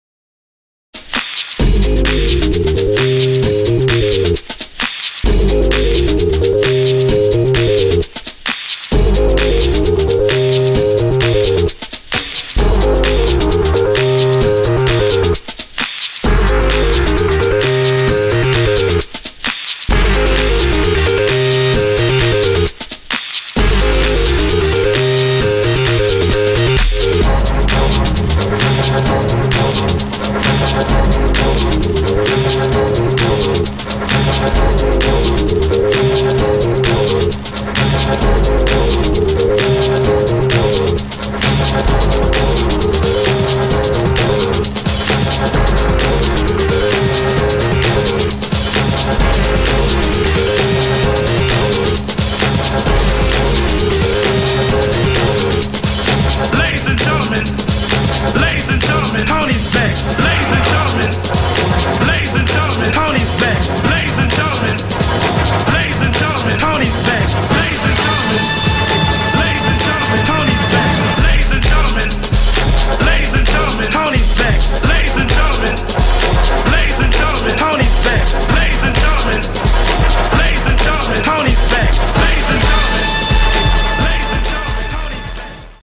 BARTH Fast-free-style
Impro graffiti 5 min / 3 couleurs / Festival Mouv'